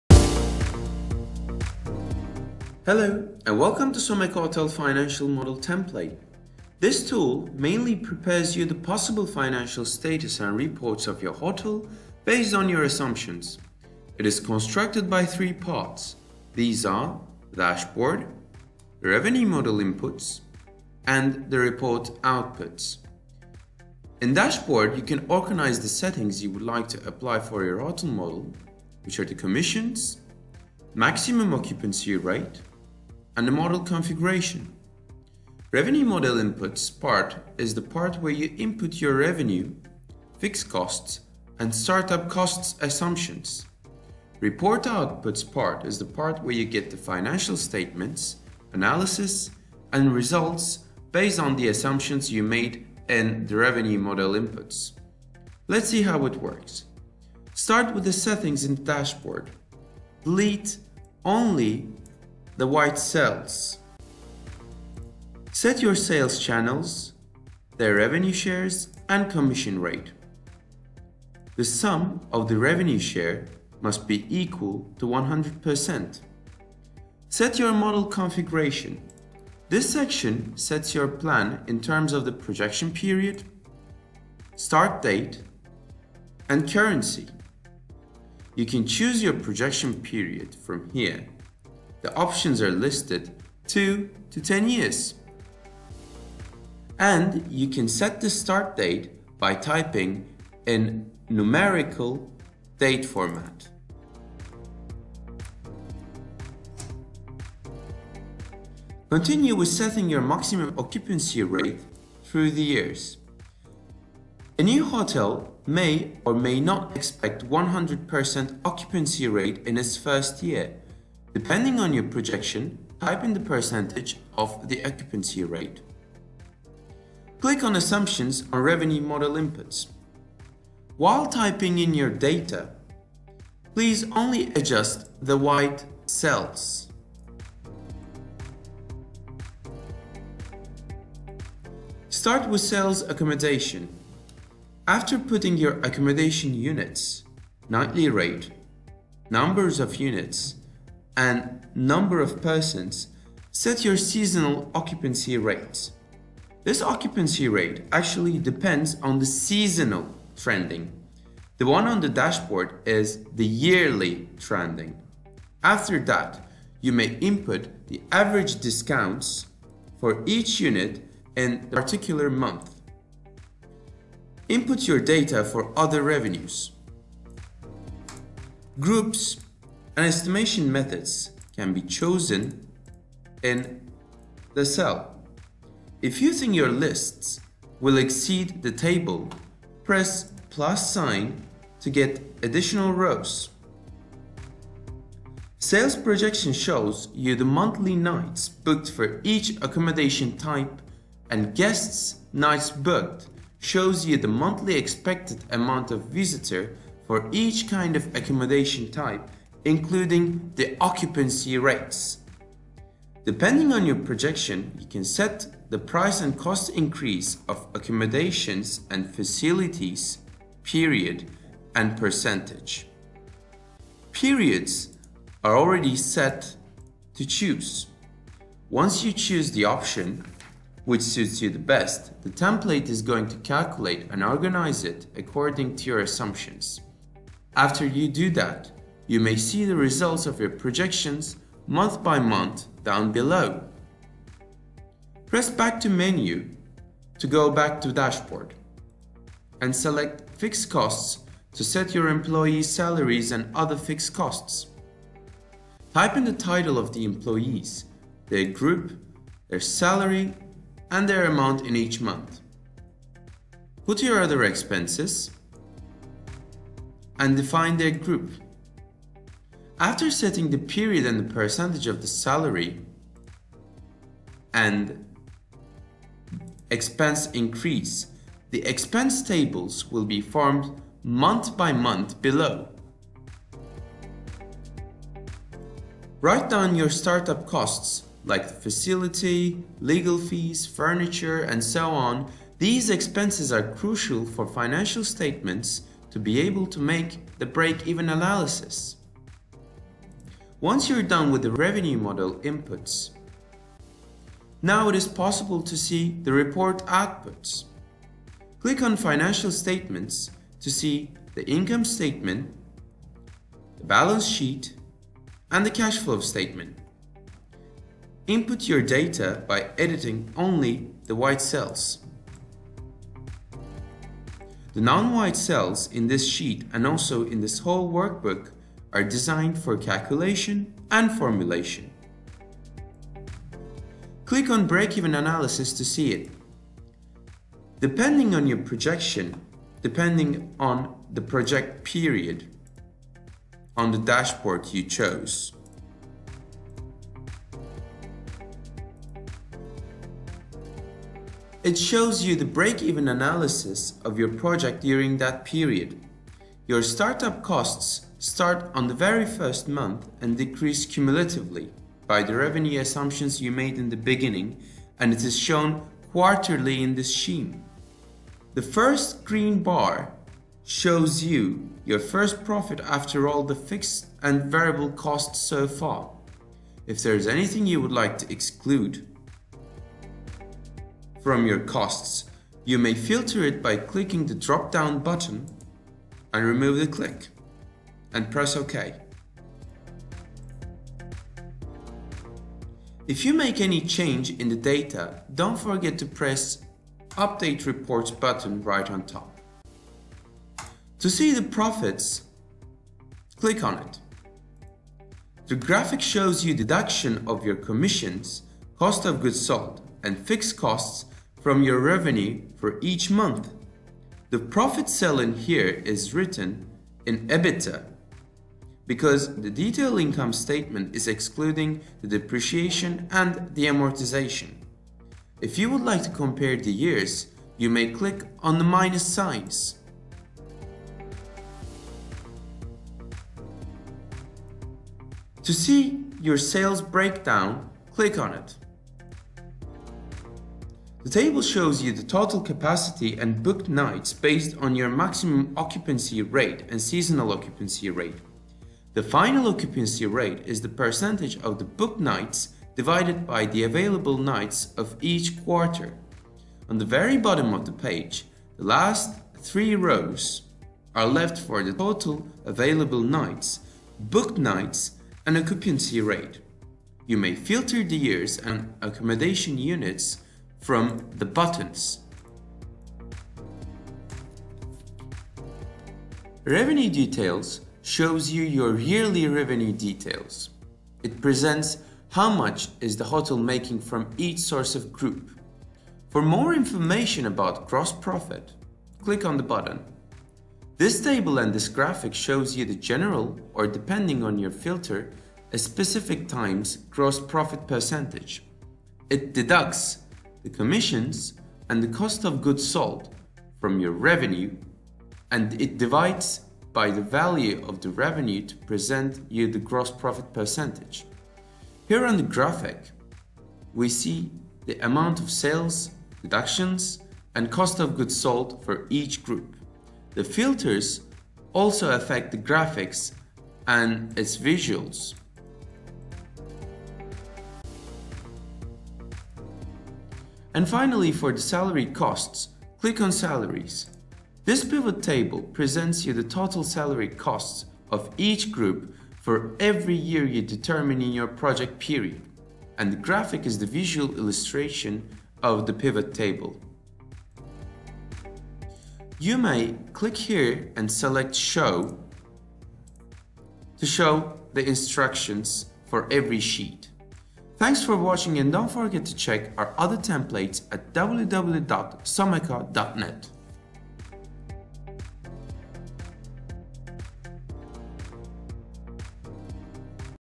Hotel Feasibility Study Excel Template in Excel
This Hotel Financial Model Excel Template will allow investors or business owners to make financial projections and decide on their hotel investment projects. With it you will:

➤ Create financial projections for up to 10 years and test them using a range of assumptions;
➤ Account for various unit types, taking into account seasonal change and year-to-year fluctuations of prices and occupancy;
➤ Analyze for different sources of income, such as accommodation, food and beverages, retail and use of amenities;
➤ Manage different sales channels, such as direct reservation, booking engines and travel agents.

Sales And Pricing Assumptions
Fixed Costs
Start-up Costs
Report Outputs:
 - Financial Statements
 - Profit (EBITDA)
 - Break-Even Report
 - Sales Report showing break even of occupancy data;
 - Detailed Revenue report presenting income by unit types and facilities;
 - Gross Profit report for each income line, and overall;
 - Salary Breakdown report

Hotel Financial Model Template Features Summary:
# Unique Excel Template to perform hotel financial feasibility study
# Professional design and suitable for presentations
# Innovative reporting system
# Financial Statements
# On Sheet Instructions